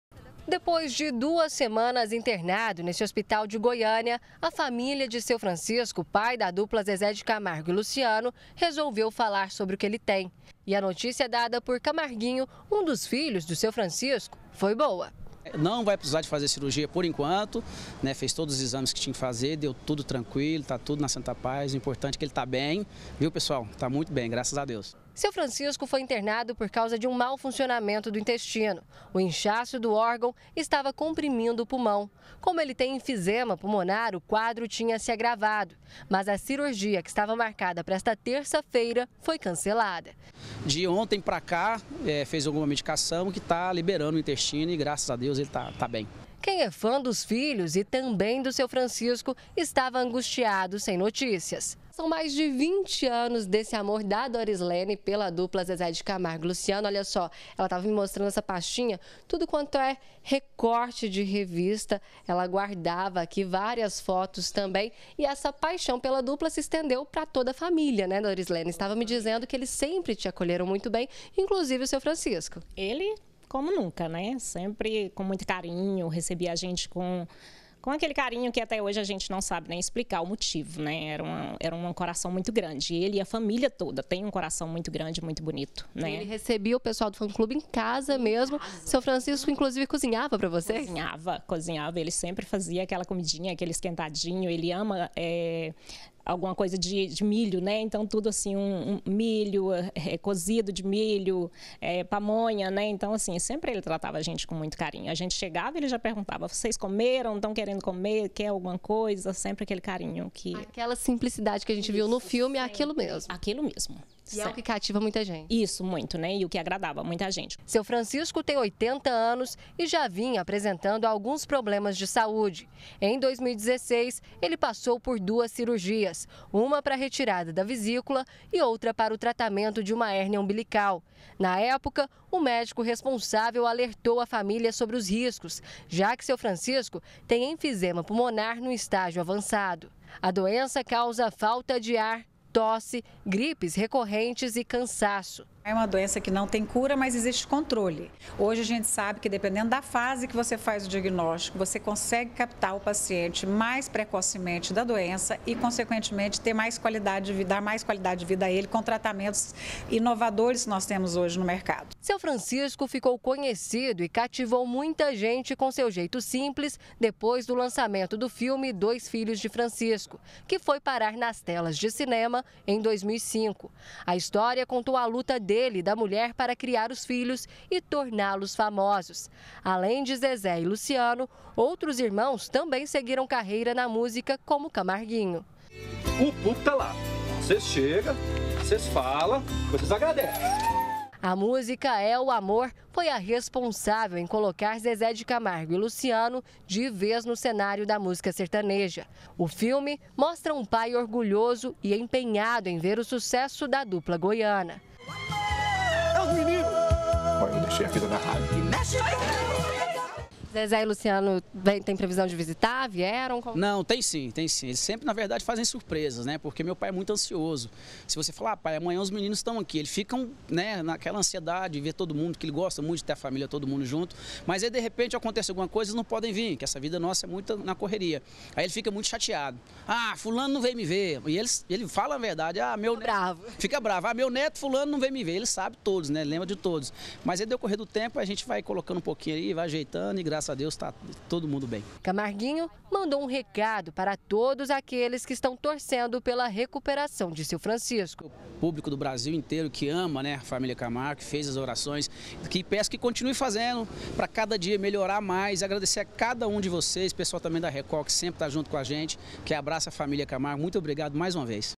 BG - Pai de Zezé e Luciano já está melhor - 24-04-2018
Depois do susto pela internação do senhor Francisco, agora vem a boa notícia. Em entrevista a repórter Patrícia Piassa, o filho Camarguinho tranquilizou os fãs sobre o estado de saúde do pai. Confira.